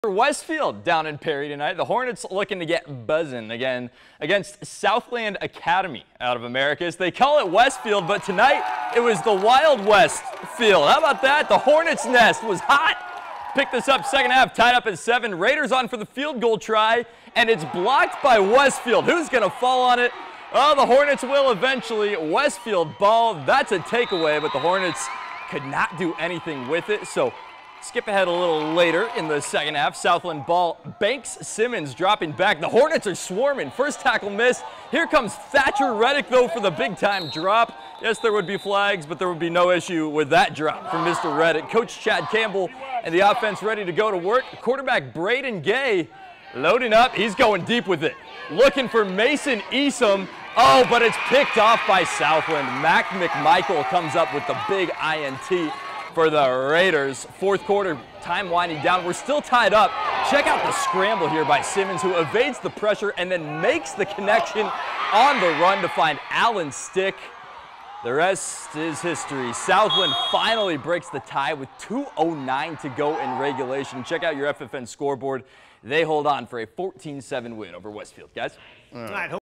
Westfield falls in close game to Southland
The Hornets offense couldn't find a rhythm and dropped to 1-2 on the season.